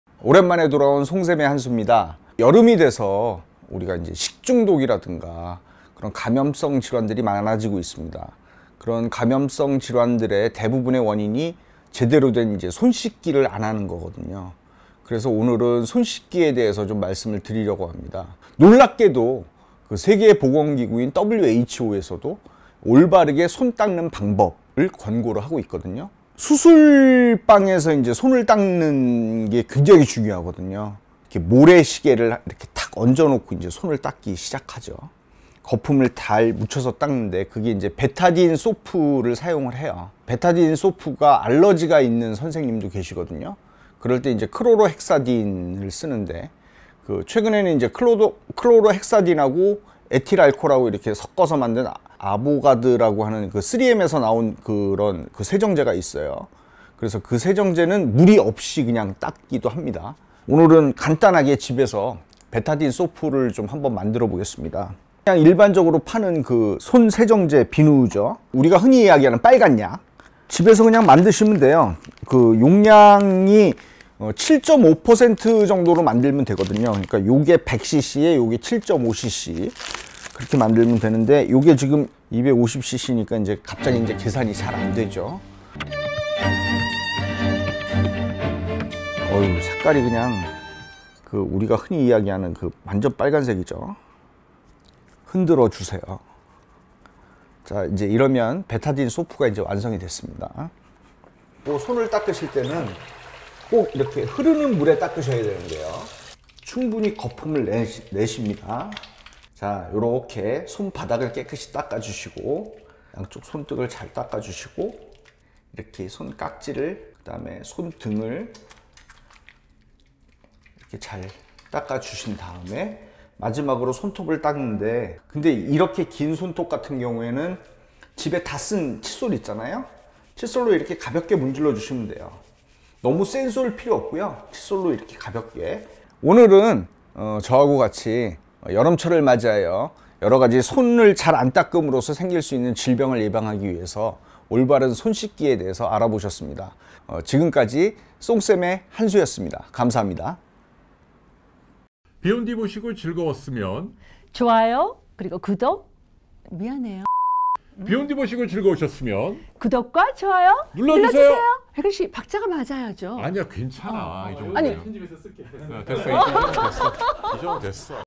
[2분30초] 의사들이 수술실에서 사용하는 비누 집에서 만들어 쓰자 - 송내과의원 송태호 원장
여름철에는 식중독등 감염질환이 급증합니다. 의사들은 손을 잘 씻는 것 만으로도 감연질환을 예방할 수 있다고 합니다.
그렇다면 감염으로부터 100% 보호 받아야 하는 수술실에서는 어떤 비누를 사용해서 손을 씻을까요? 비온뒤 멘토 동네의사 송태호 원장이 집에서 간단하게 만드는 방법을 알려드립니다.